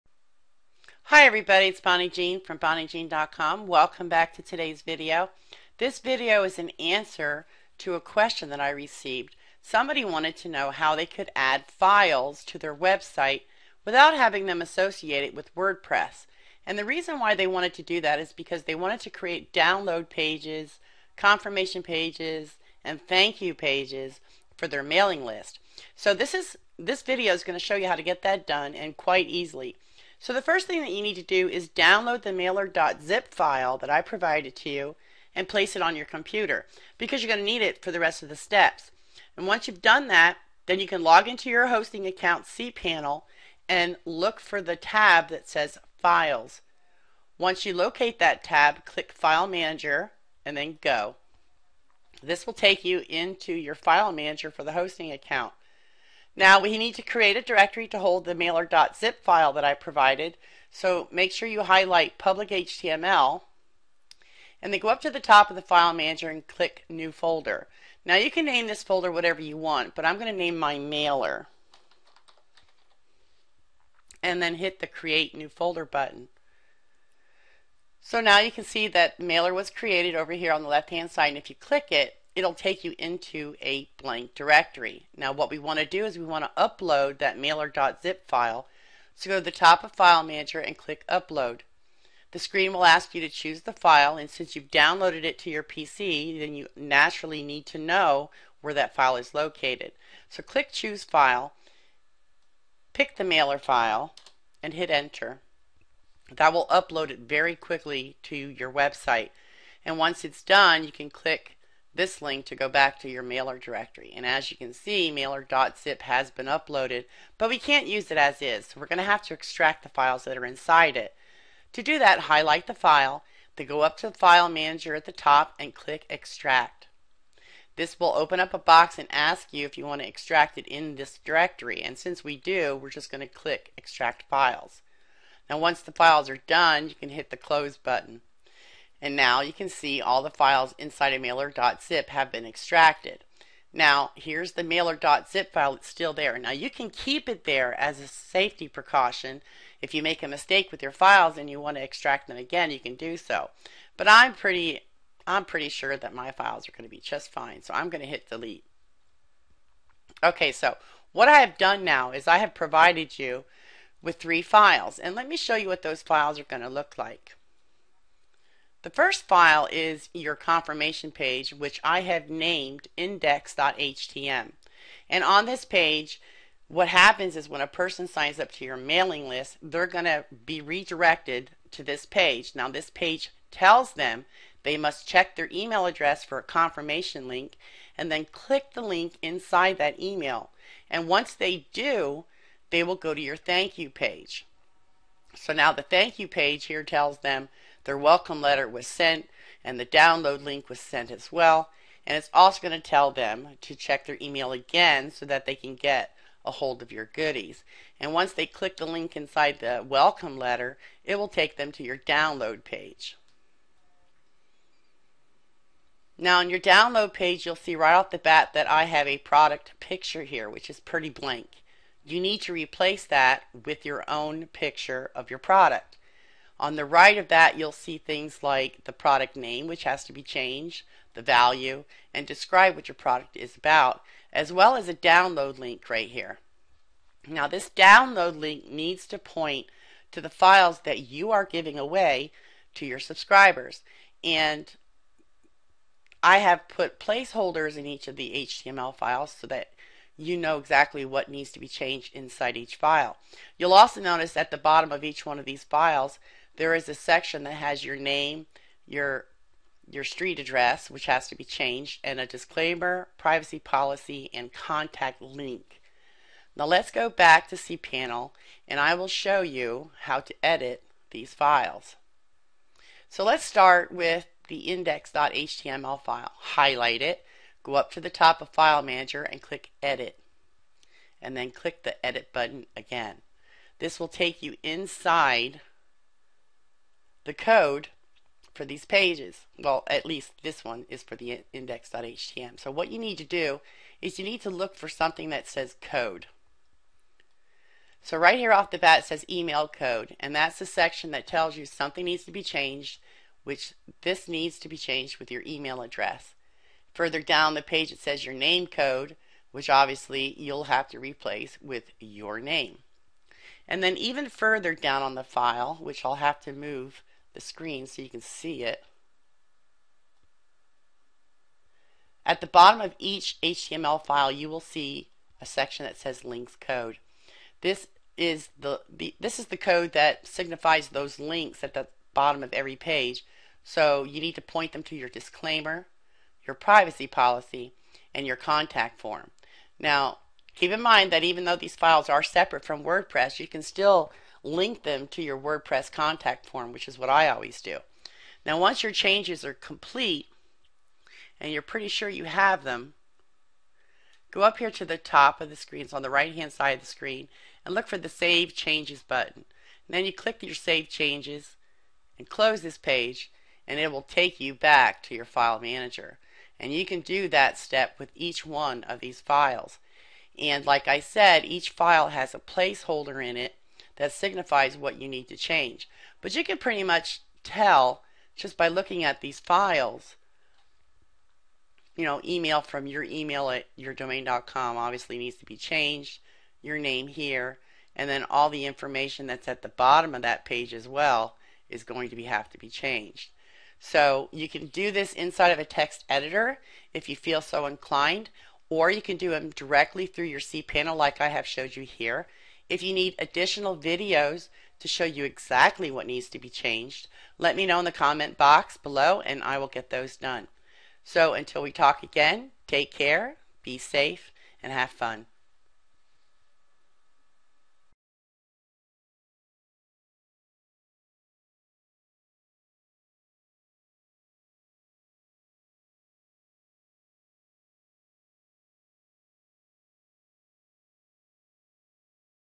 How to Add HTML Pages Outside of WordPress
- This video explains how you can add extra HTML pages to your website without needing to add them to a WordPress blog.